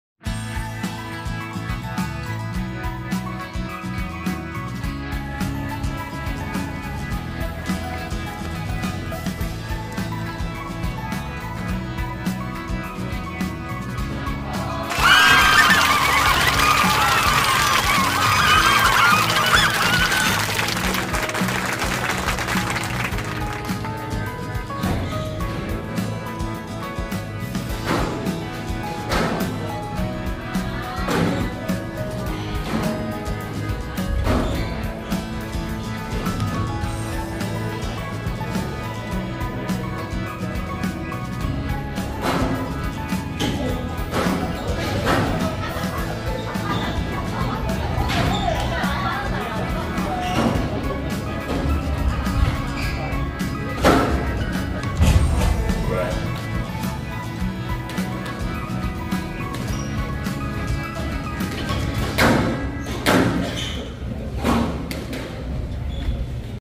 제16회 회장배 스쿼시 동호인대회 대회준비 완료했어요...
저는 출전하지않지만  전국대회라니 설레네요